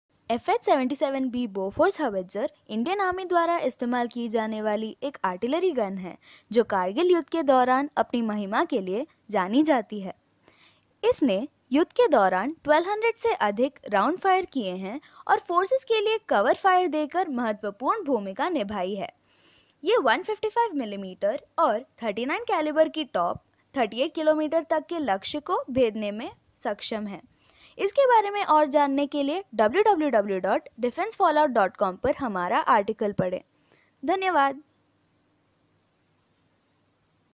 Bofors Gun System India
Know about Swedish Bofors Gun System which helped India During Kargil War